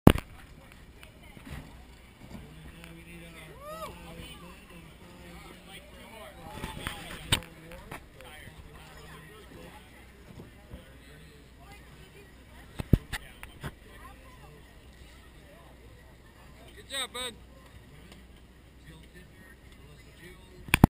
Keith Then Logan: Backflips Into Airbag
Backflips into giant airbag at the Sea Otter Classic 2012